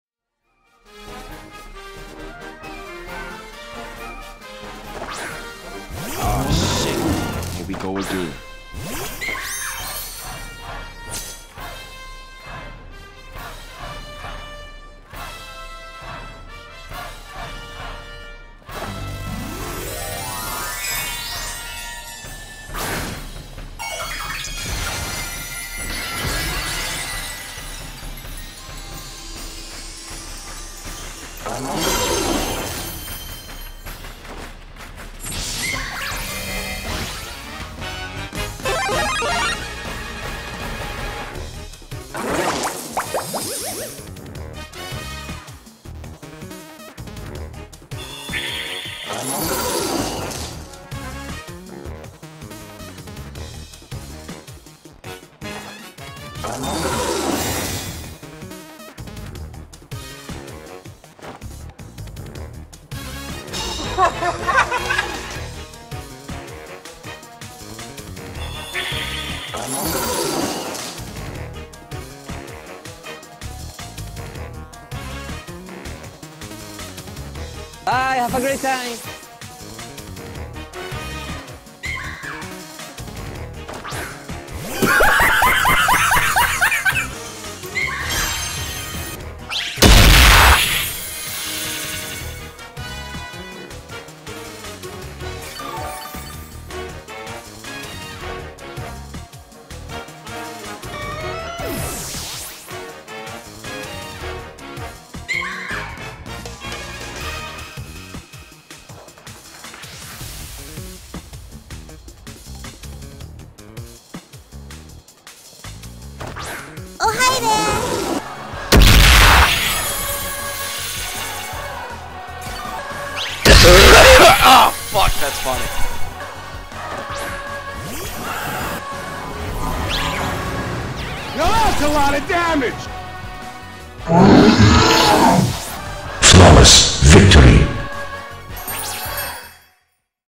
Mimikyu is so crazy...
I didn't even switch out or anything; Mimikyu just showed them who's boss.